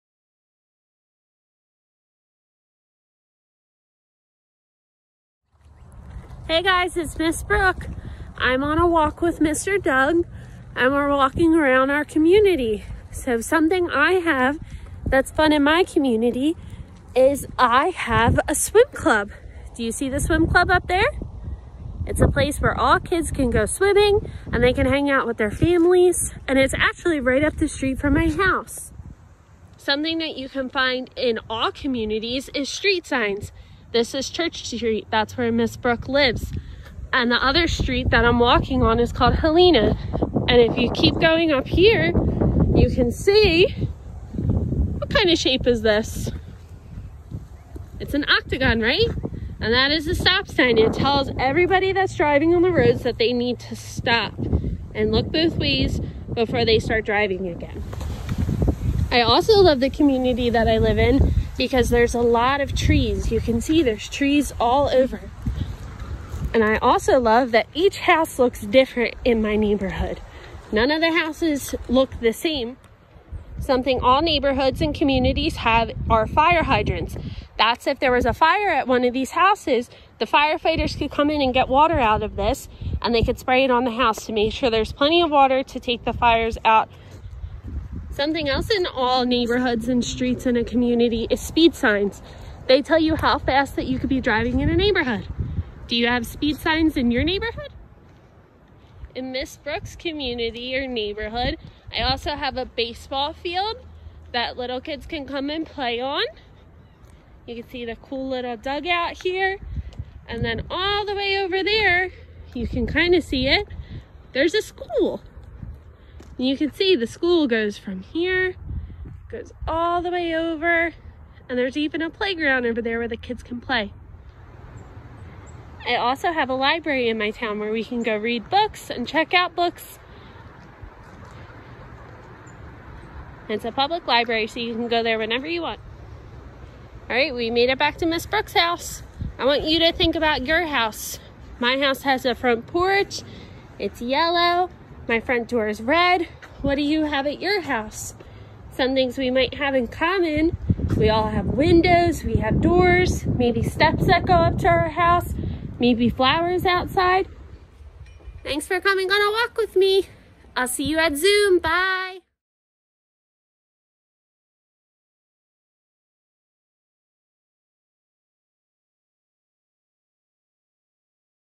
Week #8 Distance Learning: Taking a Community Walk
In conjunction with our Distance Learning Plan for week #8 of our COVID-19 closure, Miss Brooke uses her five senses to explore the people, places, and things that make up her local community. Can you find things in Miss Brooke's community that are similar to what you see in your own?

For more information on Chesterbrook Academy of Washington Township, please visit our